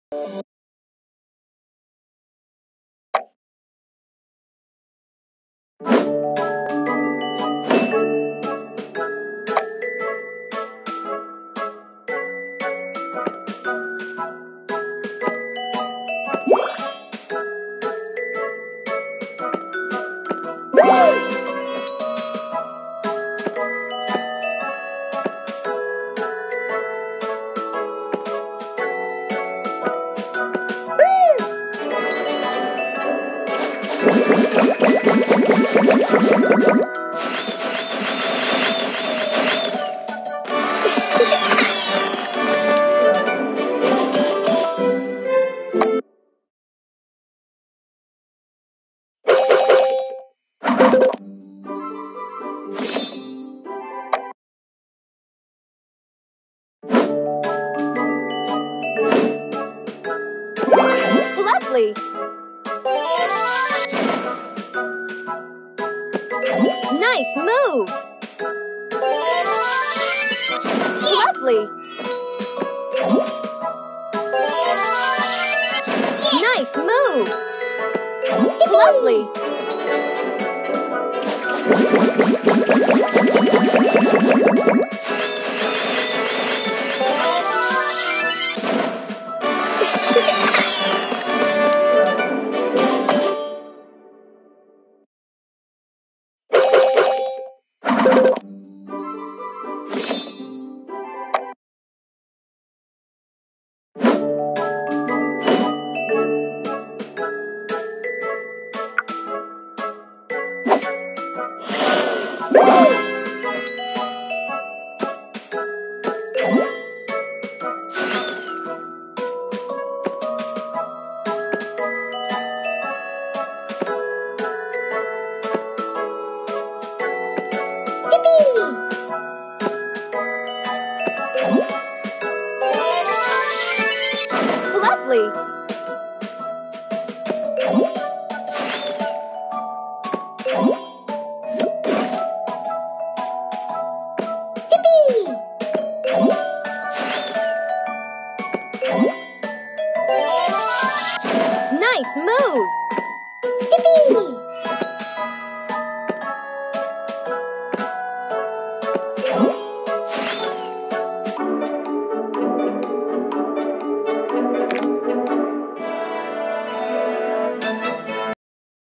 Toy game | colourful Toys for kids | colourful candy game
In this video we are playing toys game which is made for kids in this game there are colourful cubes which we have to match. To pass the level we have to match same colour cubes with each other